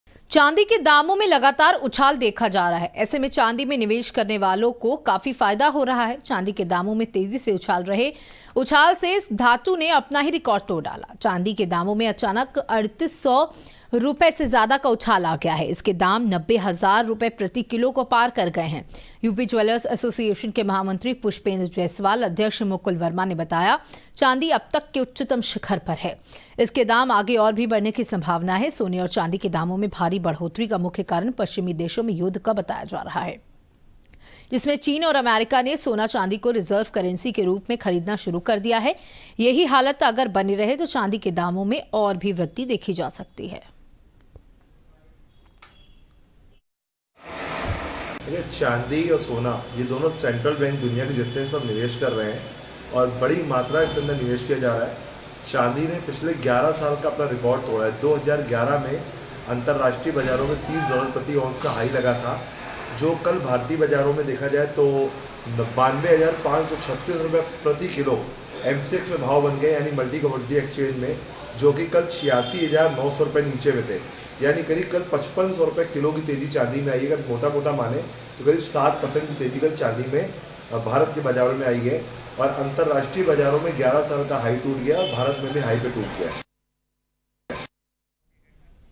कानपुर : चांदी के दामों में आया रिकॉर्ड तोड़ उछाल, 90 हजार प्रति किलो के ऊपर पहुंचे चांदी के दाम
DEN- 157
SITI –85
JGD-99